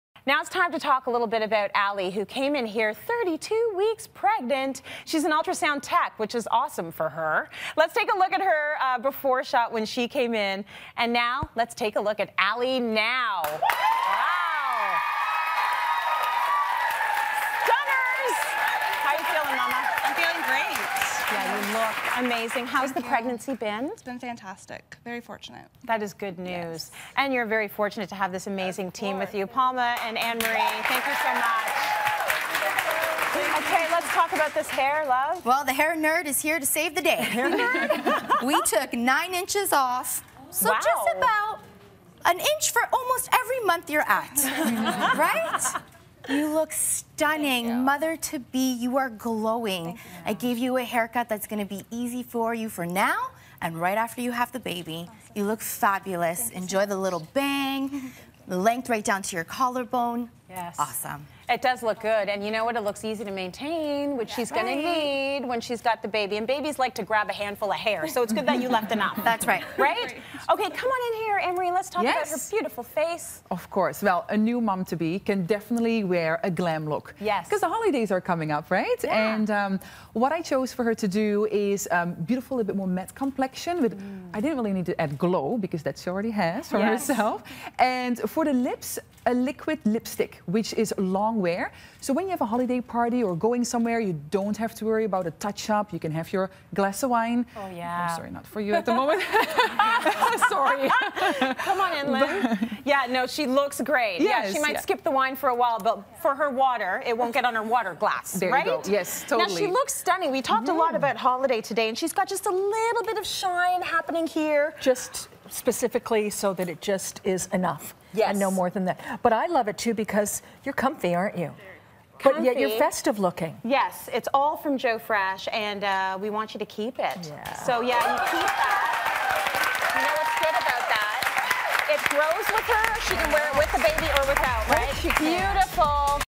Glowing mom-to-be gets a glam holiday look
Hairstylist Palma N'Sheluvzit, makeup artist Annemarie Van Meer and stylist Lynn Spence give Allie, a pregnant ultrasound tech a vampy glam holiday look.